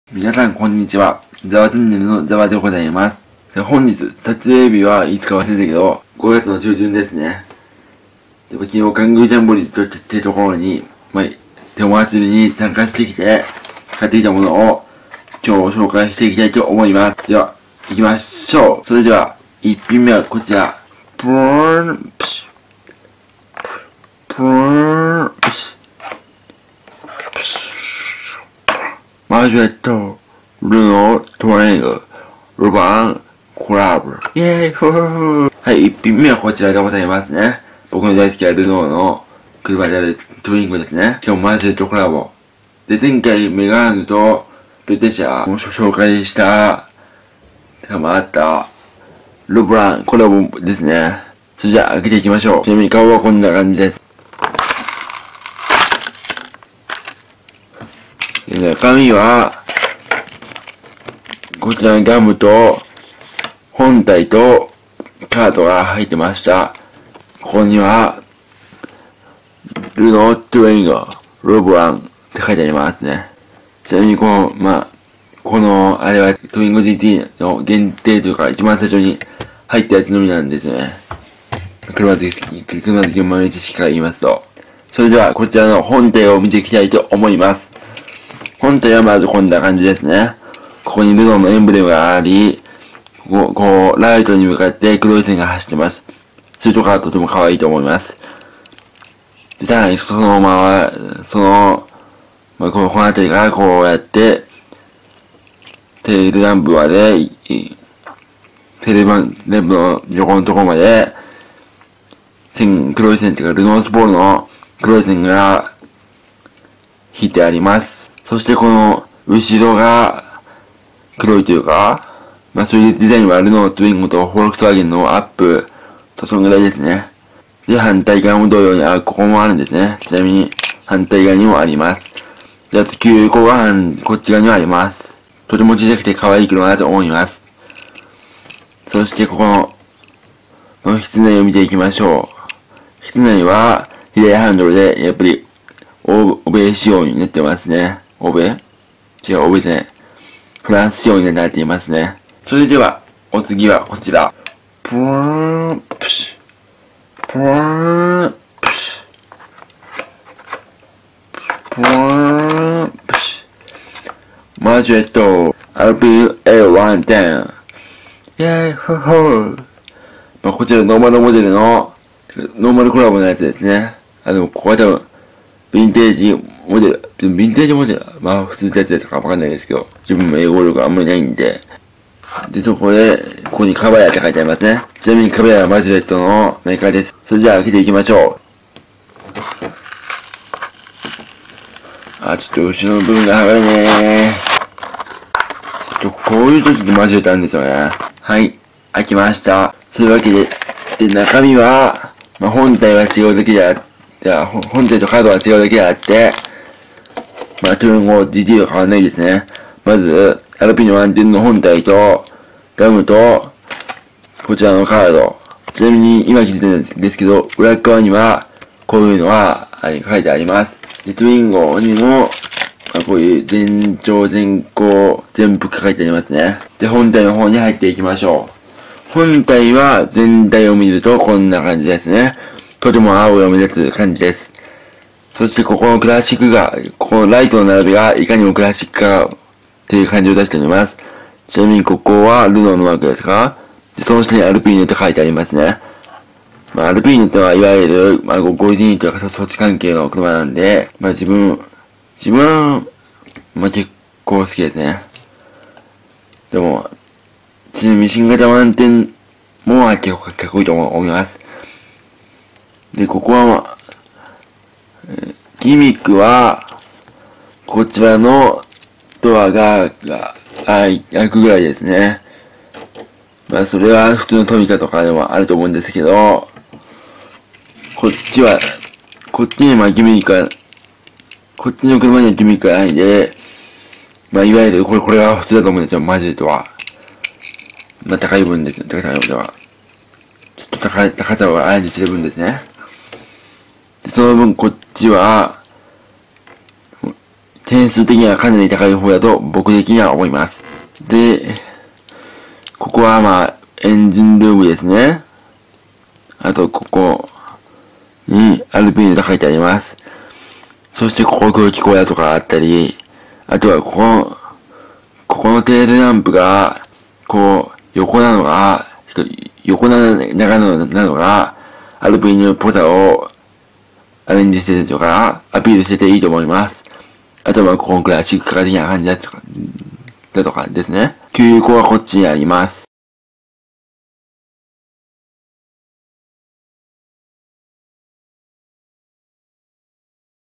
カングージャンボリーで買ってきたものを紹介するよ！ 前編
今回は、ルノーカングージャンボリー2019で購入したミニカーを紹介します